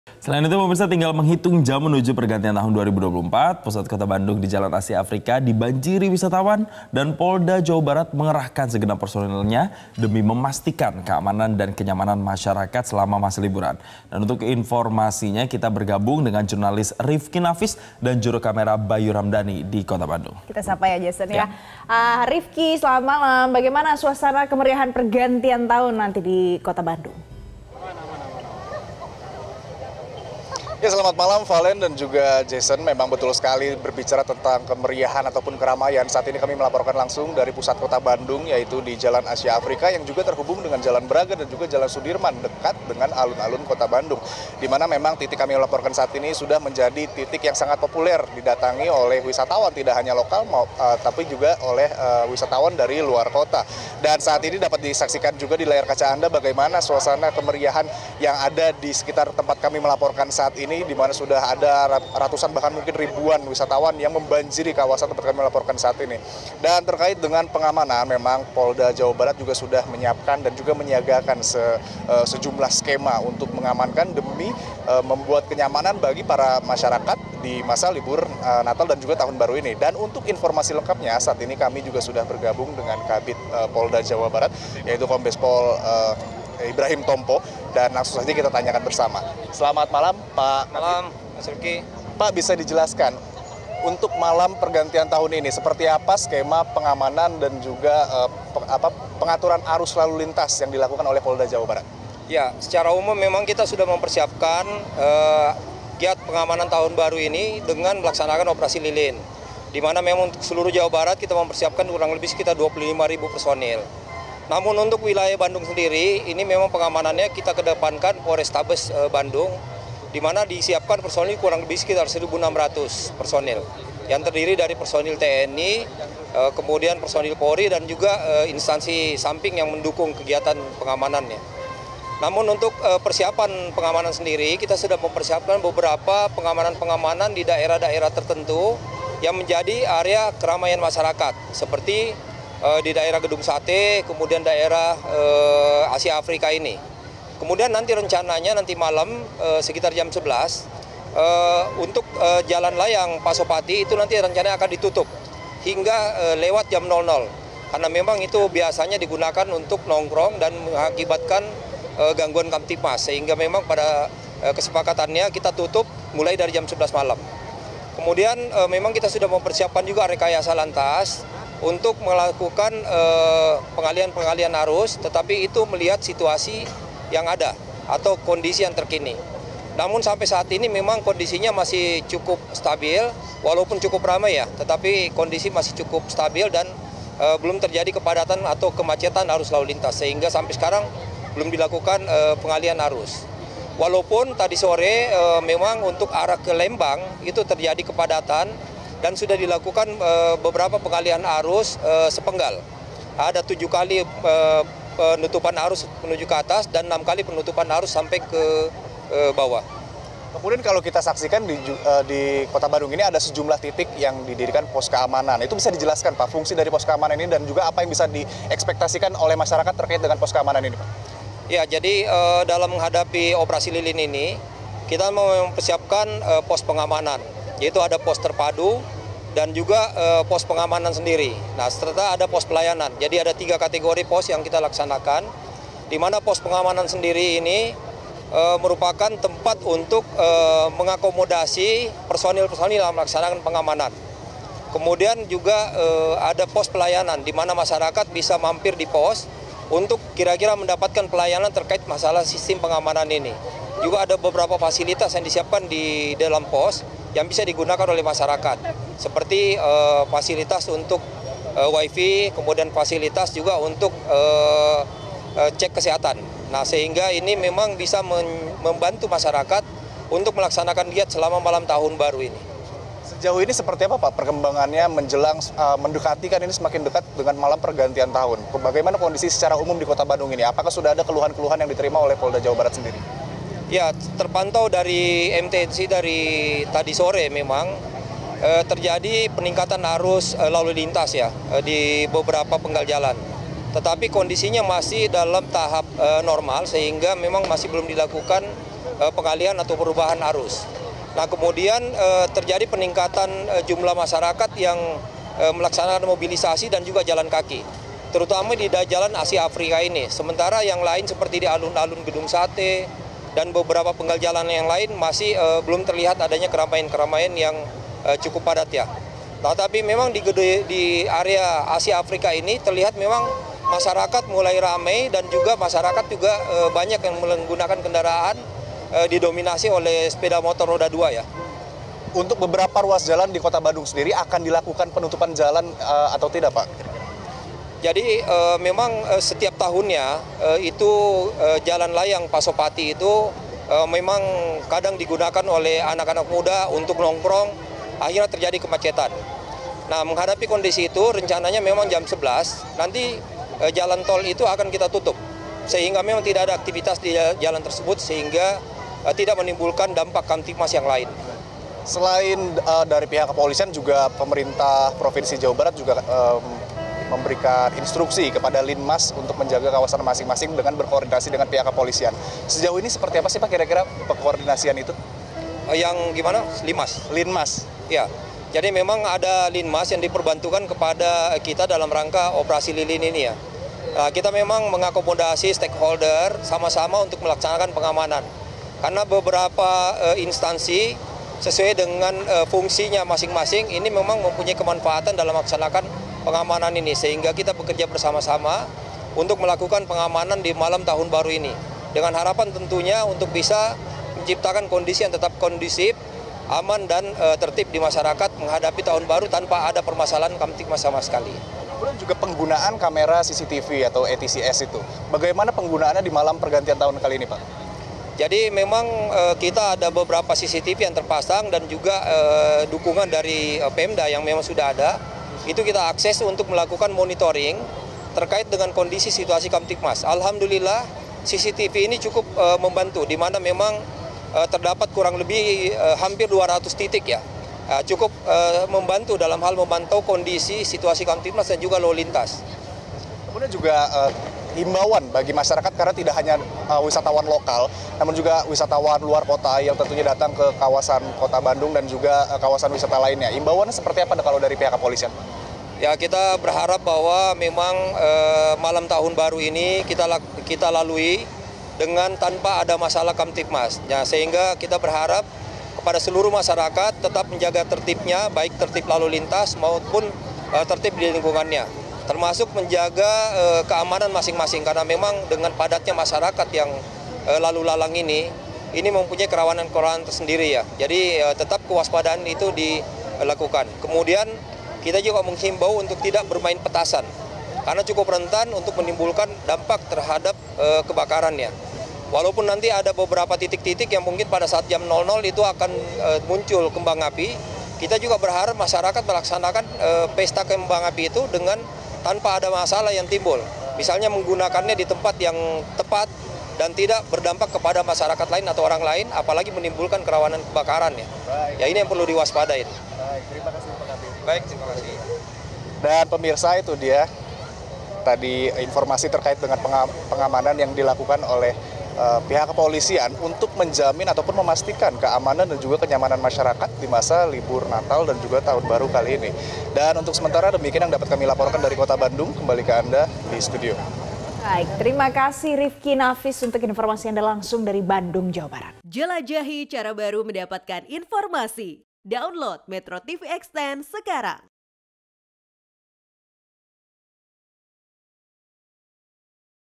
Perayaan Malam Tahun Baru 2024, Polda Jabar Jaga Kenyamanan dan Keamanan Warga
MetroTV, Kepolisian Daerah Jawa Barat akan menggelar operasi Lilin Lodaya dalam rangka pengamanan Natal 2023 dan tahun baru 2024.